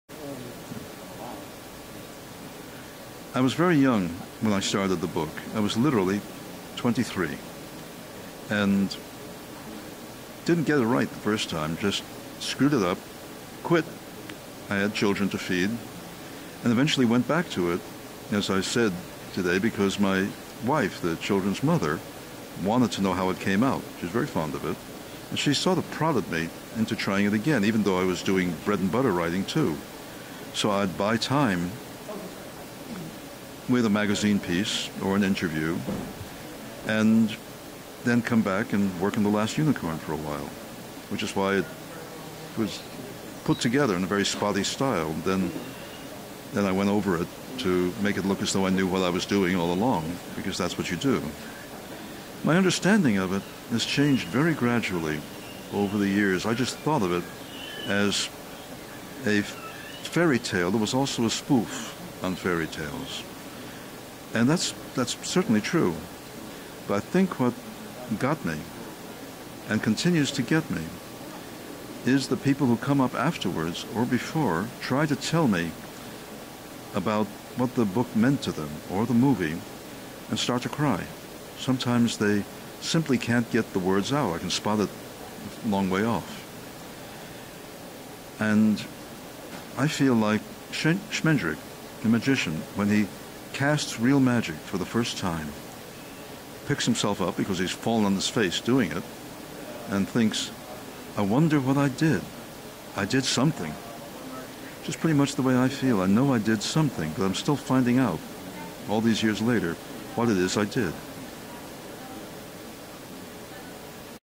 Peter S. Beagle Interview - His Evolving View Of The Last Unicorn
The book The Last Unicorn has been in Peter S. Beagle's life for over fifty years. He discusses how his view of the book has changed, as he's heard about the impact it has had on readers' lives.
Interview conducted at Bay Park Square Cinema in Green Bay on September 21st, 2014.
Video courtesy of WCWF CW 14